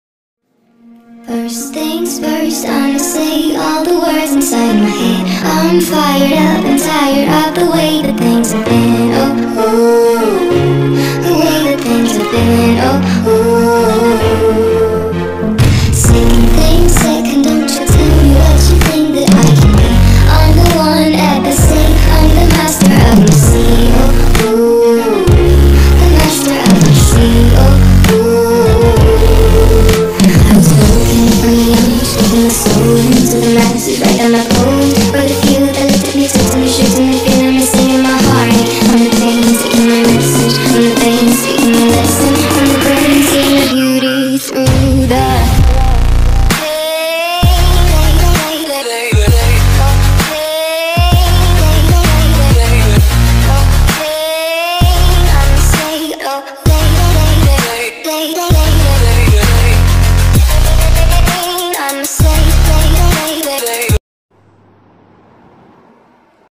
Rouge mortis skin and game play
I bought rouge mortis skin😎 ,and some game play with the skin .